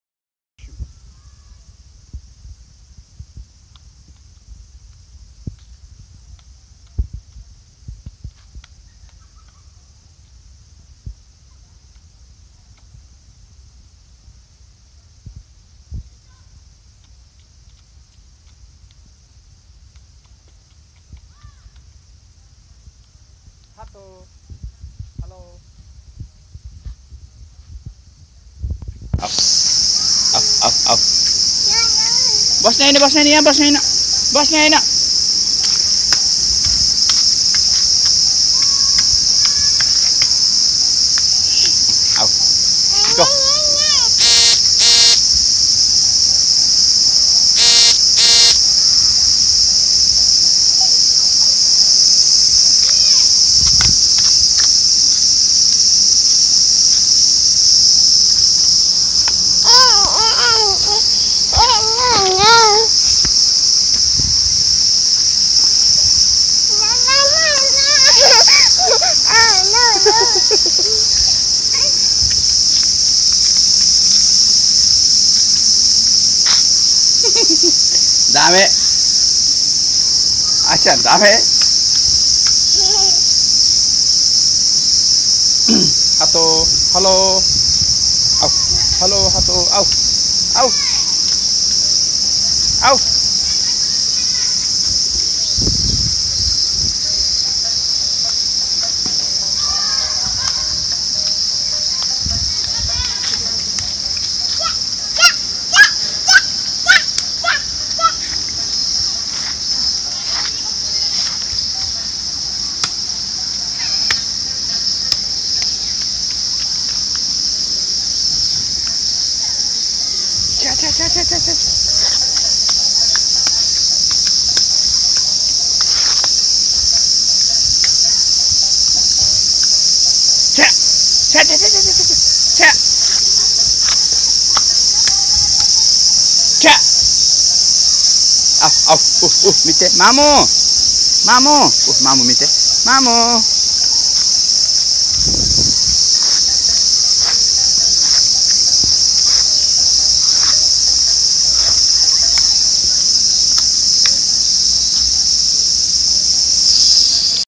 Asuka playing in water
Asuka always be happy when she get freedom in ranny time
Funabashi park